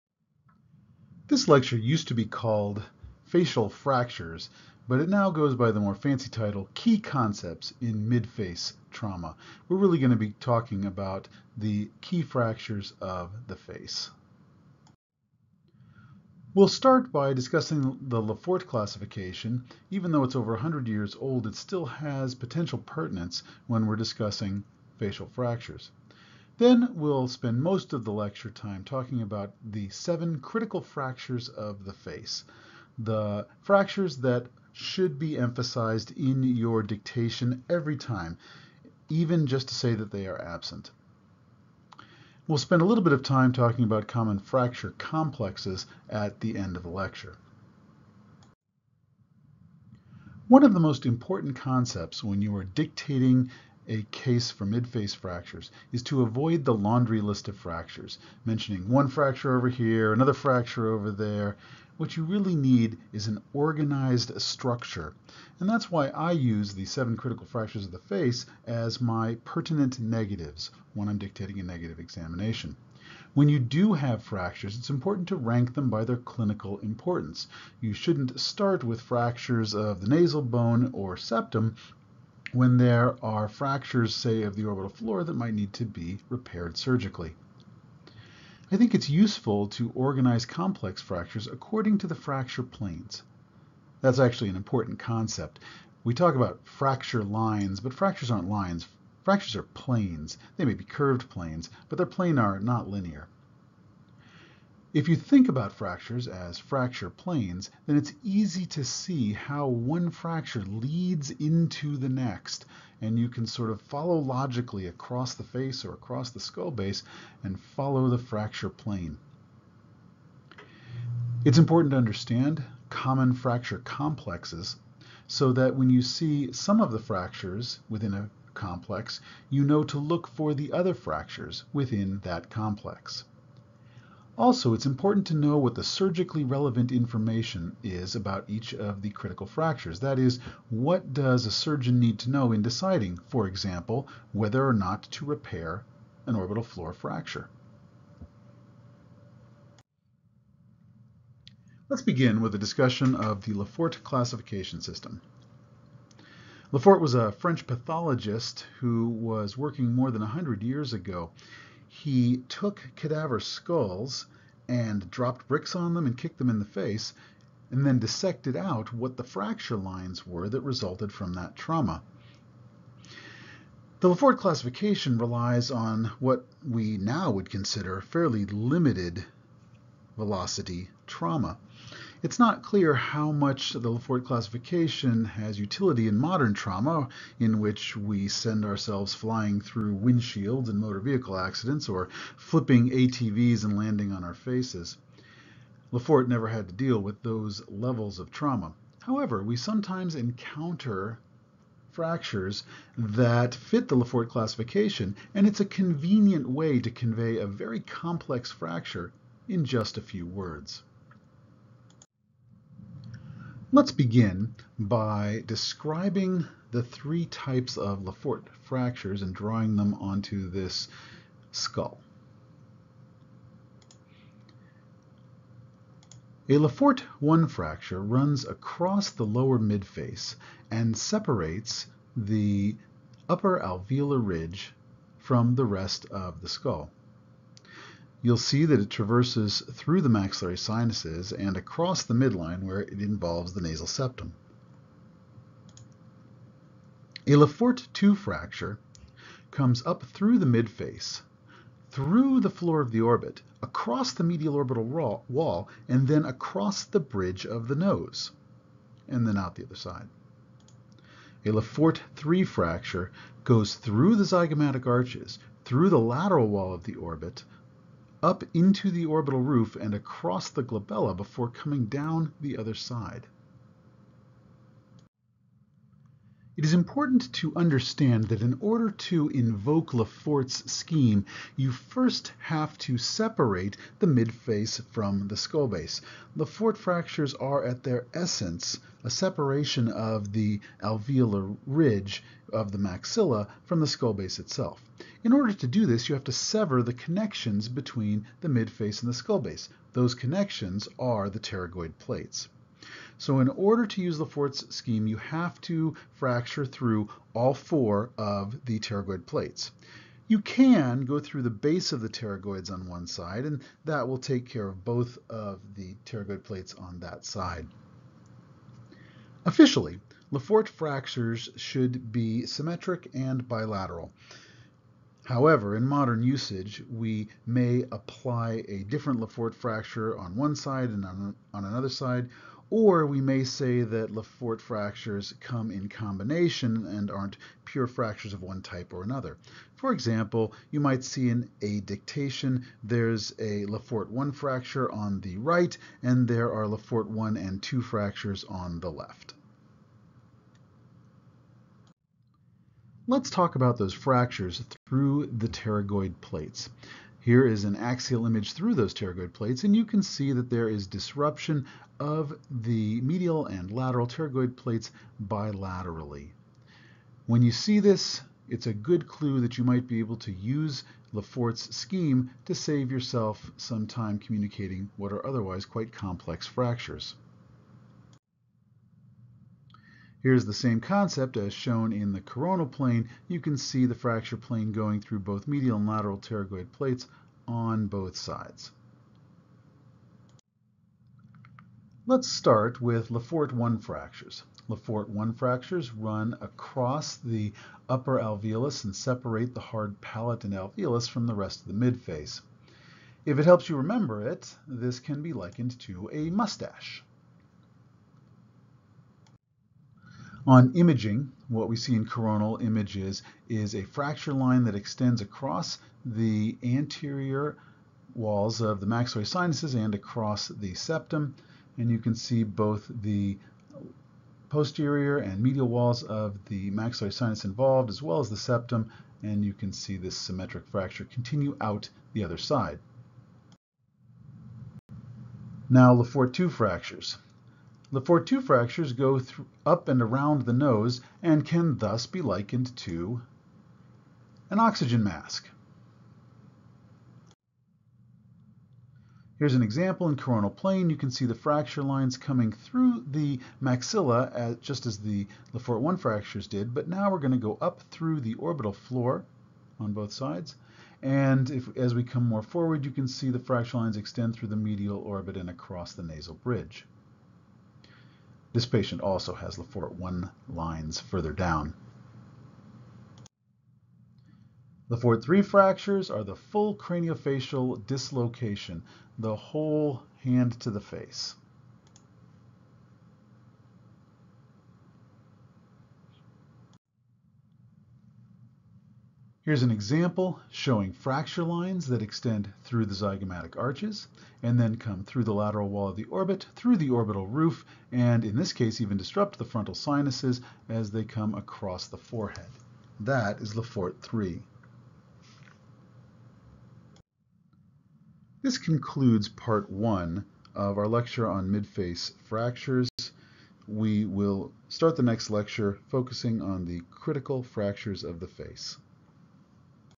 Facial Fractures 1
Part 1 of the lecture on mid-face trauma.  LeFort fractures.